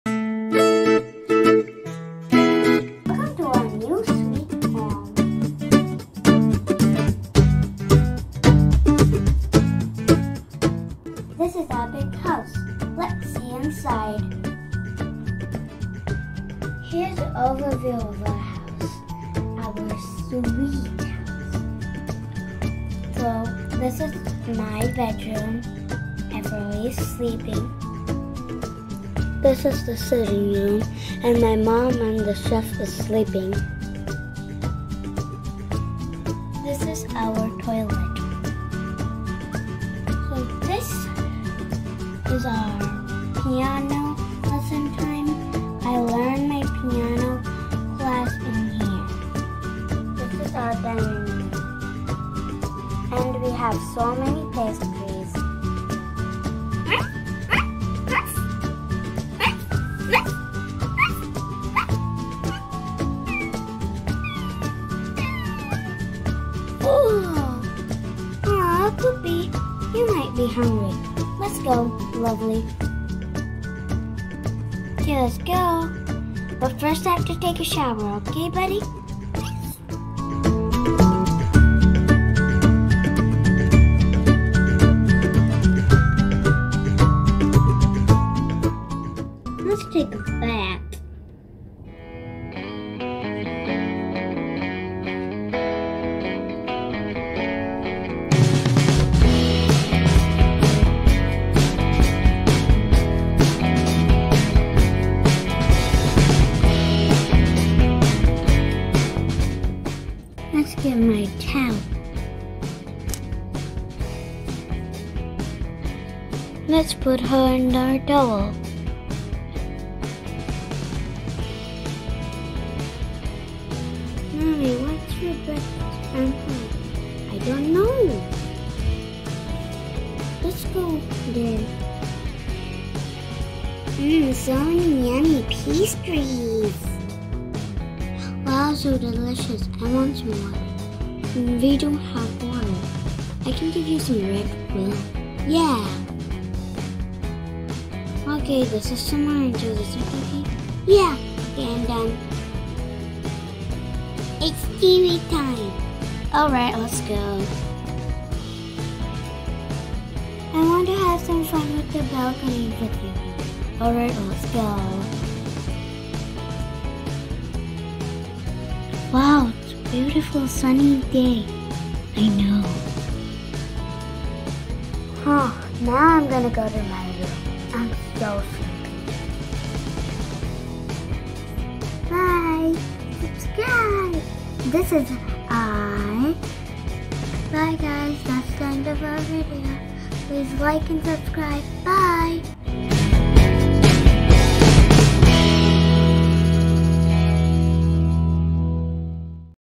Our New Sweet Home
Our New Sweet Home has a bathroom, Kitchen & Tea Pretend Play Toy Kitchenware & Food Playset. Playtime will be more entertaining with this well-furnished miniature house.